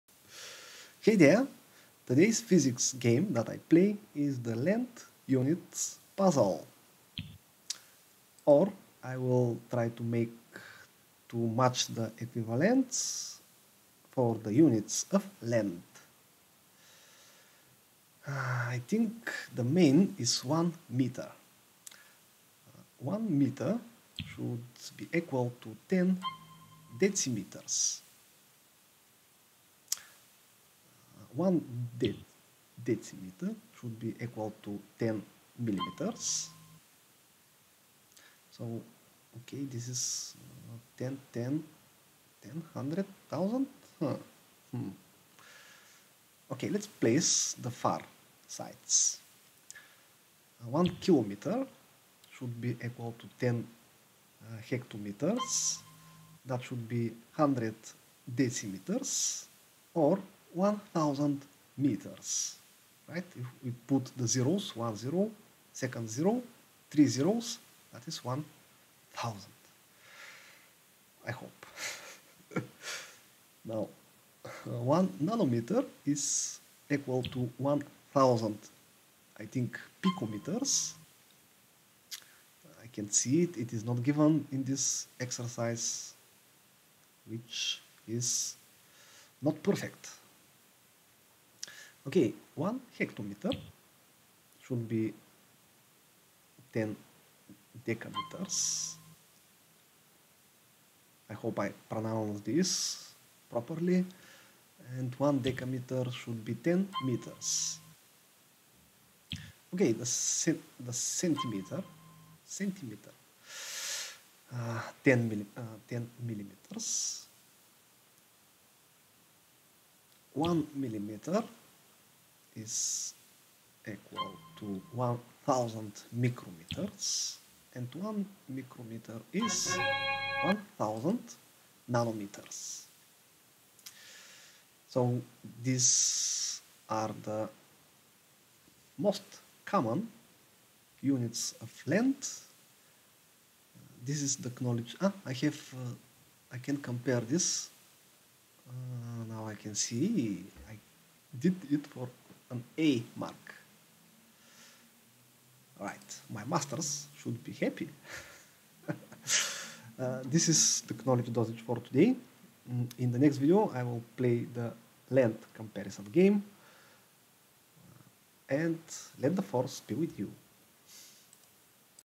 The Units of Length - Interactive Physics Lesson 🚀🎥🎓
Fun educational video about the units of Length (Metre) by their equivalents in order of magnitude. Suitable for online lessons, interactive classes and exciting homework.